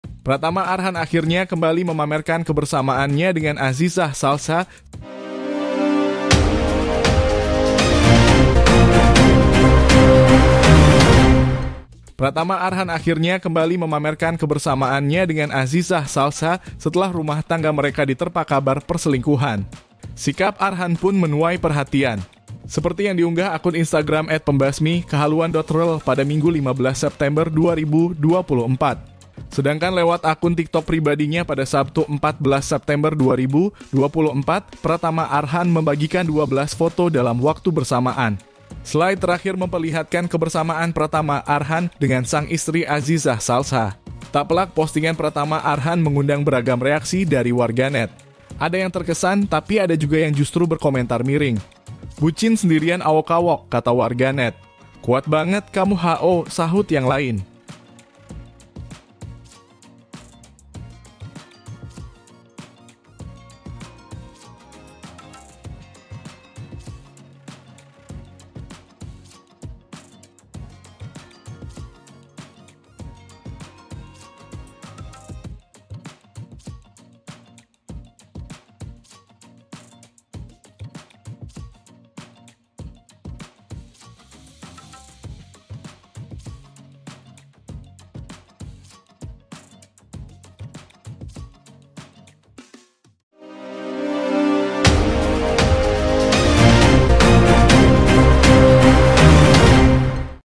Arhan Pratama Kini Dicibir Netizen Usai Posting Foto Mesra Bareng Istri
Pratama Arhan akhirnya kembali memamerkan kebersamaannya dengan Azizah Salsha setelah rumah tangga mereka diterpa kabar perselingkuhan.

Sikap Arhan pun menuai perhatian.

Seperti yang diunggah akun Instagram @pembasmi.kehaluan.rell pada Minggu (15/9/2024).

Sedangkan Lewat akun TikTok pribadinya pada Sabtu (14/9/2024), Pratama Arhan membagikan 12 foto dalam waktu bersamaan.

Slide terakhir memperlihatkan kebersamaan Pratama Arhan dengan sang istri Azizah Salsha.

Tak pelak, postingan Pratama Arhan mengundang beragam reaksi dari warganet. Ada yang terkesan, tapi ada juga yang justru berkomentar miring.